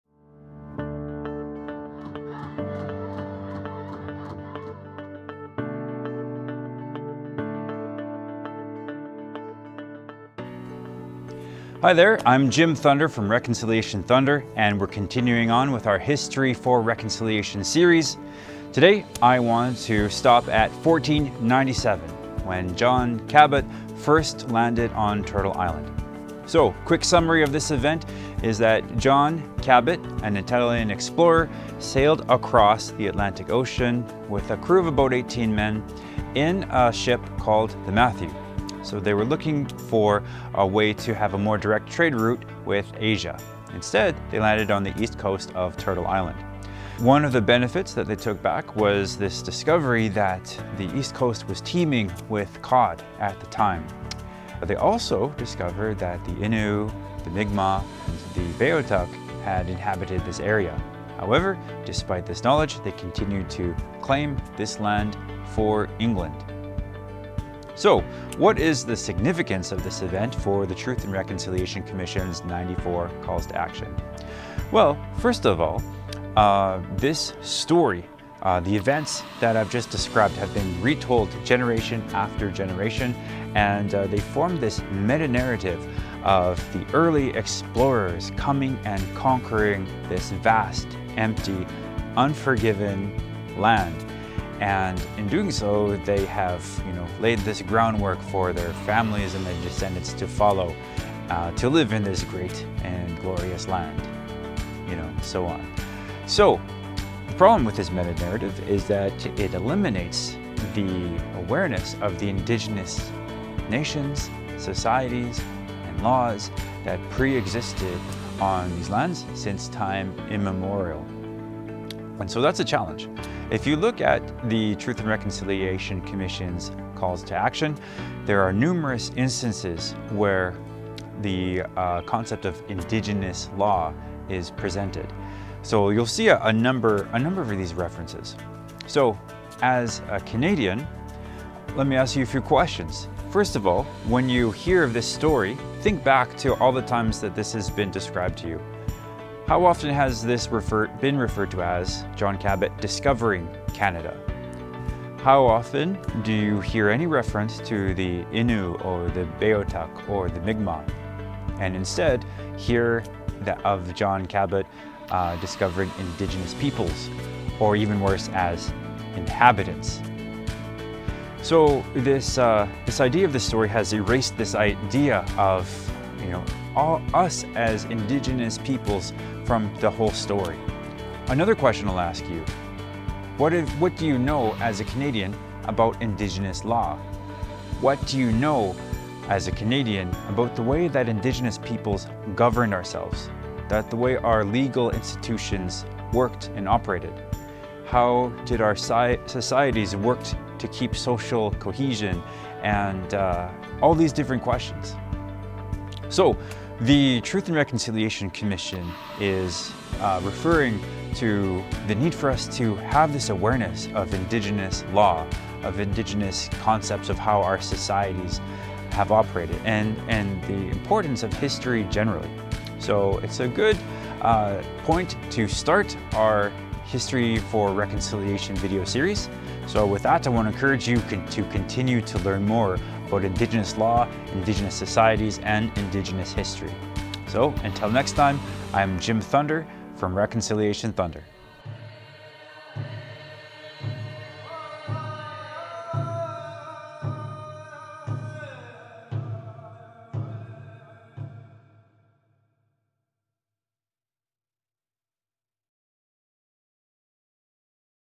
History4Reconciliation - When John Cabot discovered the New World
Continuing on with our
History4Reconciliation Campaign!

We take a look back in time to the 1497, when Italian
explorer/navigator John Cabot sailed across the
Atlantic Ocean in an attempt to create a new trade
route with Asia. Instead, they landed on the East Coast
of Turtle Island.

This event has been retold generation after generation,
creating this narative that the early explorers
conquered this vast "empty" land.

Take the time to watch this part in our series as it
explains the effects of this historical event had on the
indigenous peoples of Turtle Island!